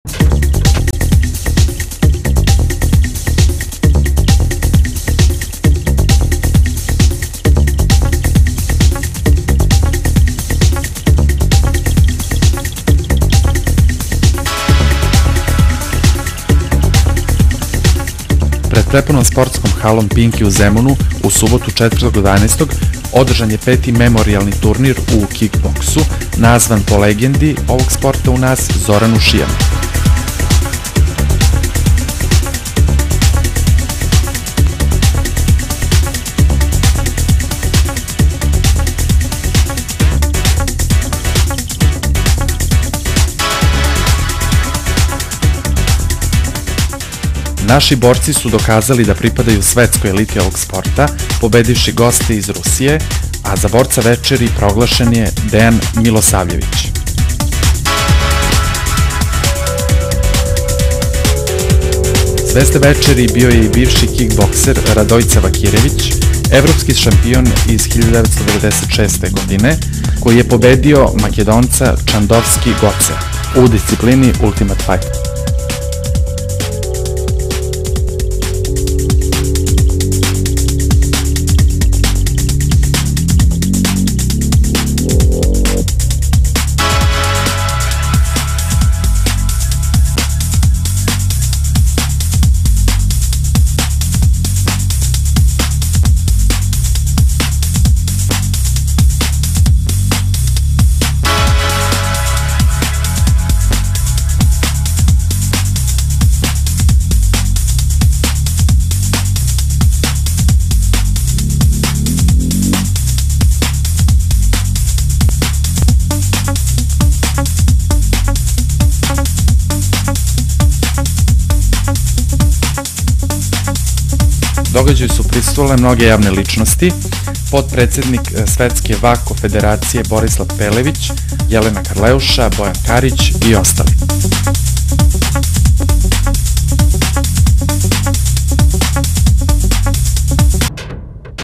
Kik boks turnir " Zoran Šijan" 2004 evropski šampion Radojca Vakirević i Dejan Milosavljević
Autor :LJUBIŠA STEFAN VELIČKOVIĆ